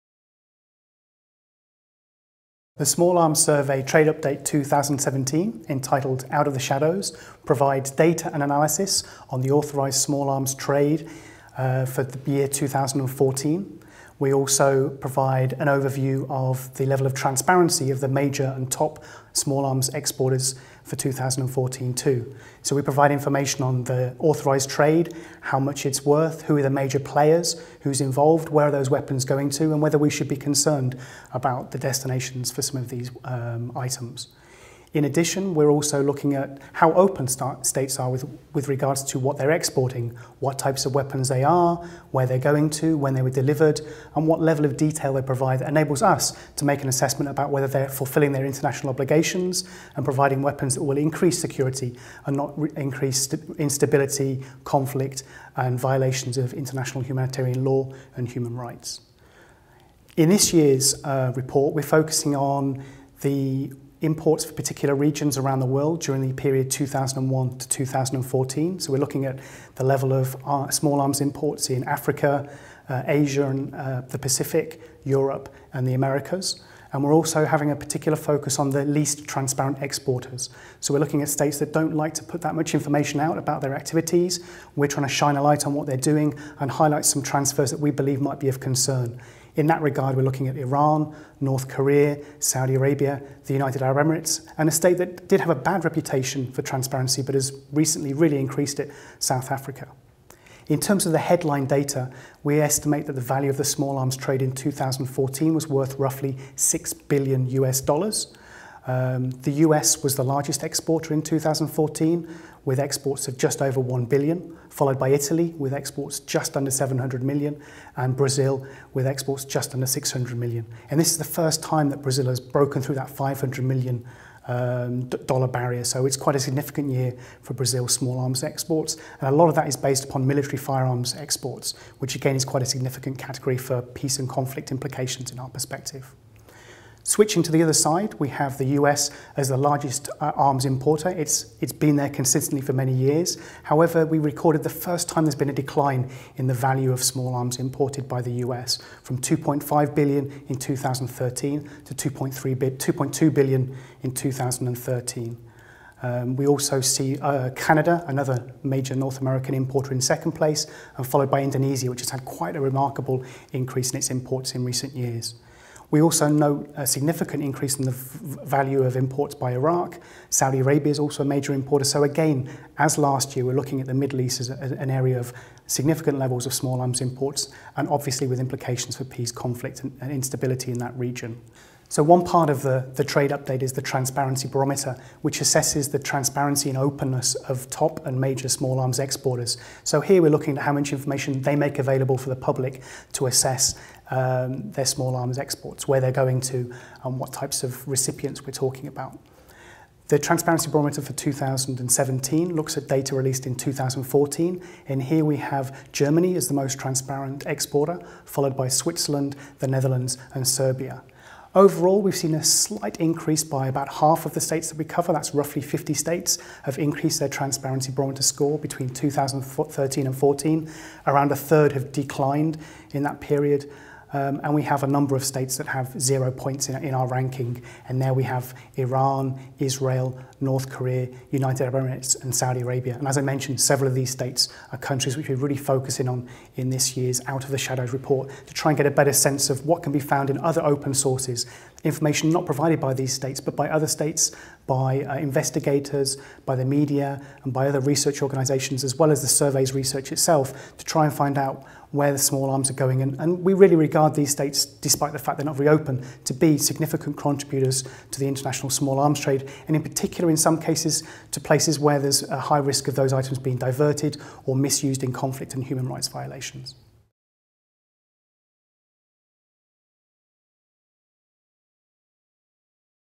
Small arms - a $6 billion global business
Paul Holtom introduces the 2017 Small Arms Survey report on global arms sales.